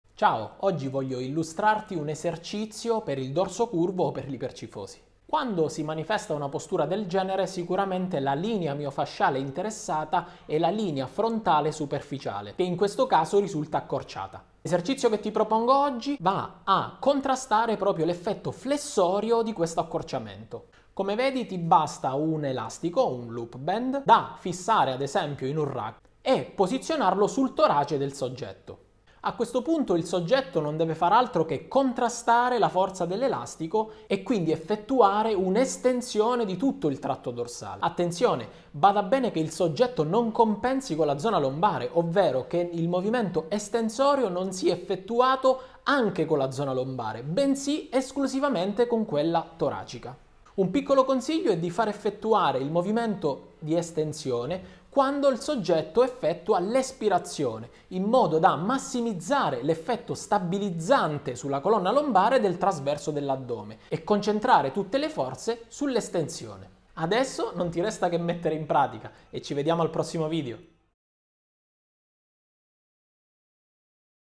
IPERCIFOSI - Esercizio di recupero
Esercizio di recupero per ipercifosi o dorso curvo